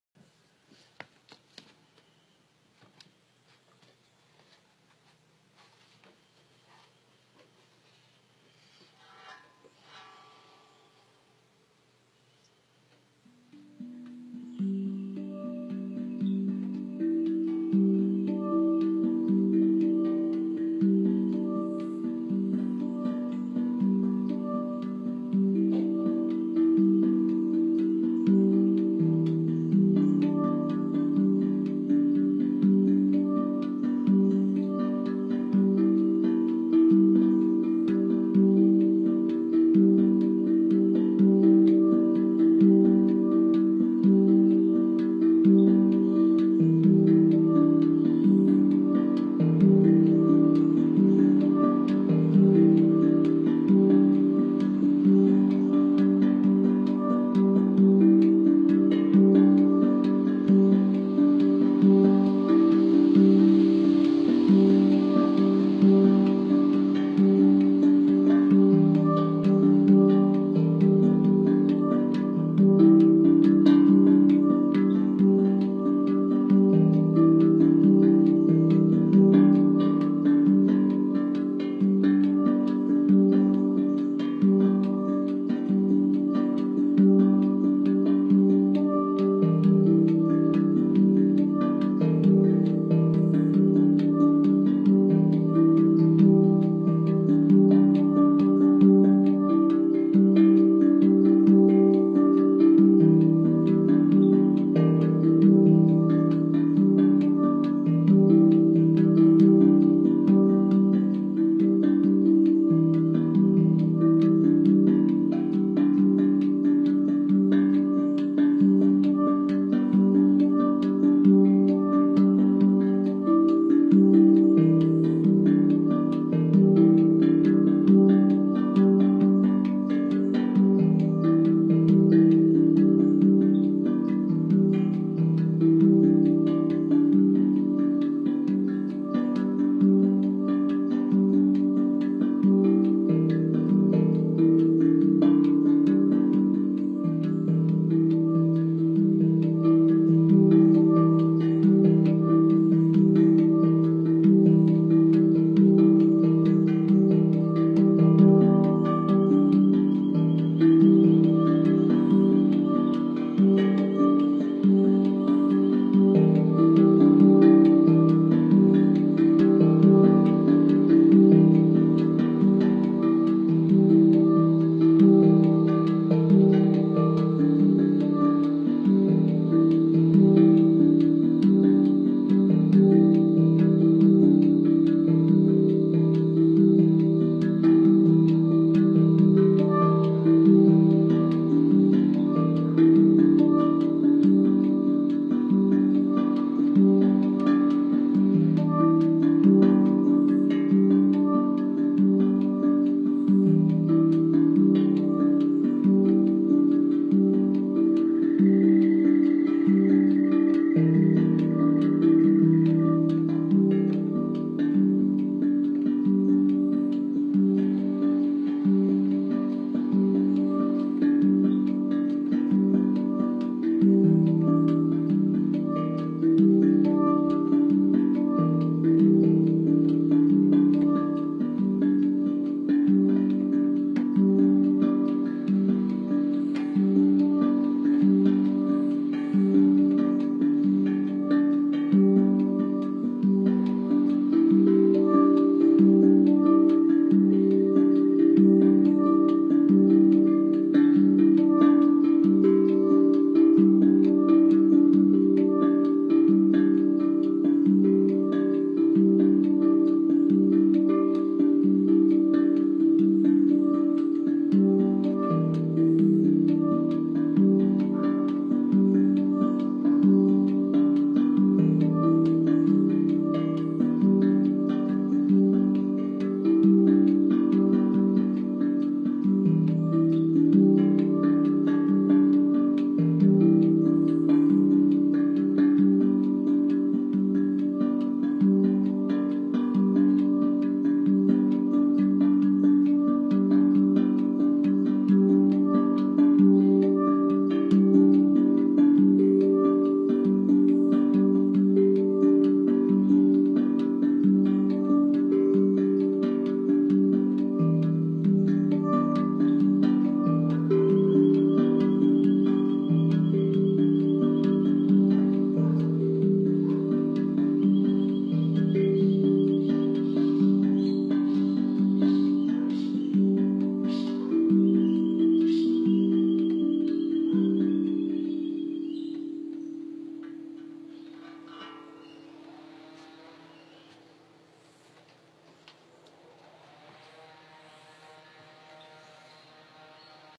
The Frequency Portal
Hand pans Pi, and Gasong, an intermitt moment listening and playing to the intricate frequencies of the land, it is a portal here, divne and true.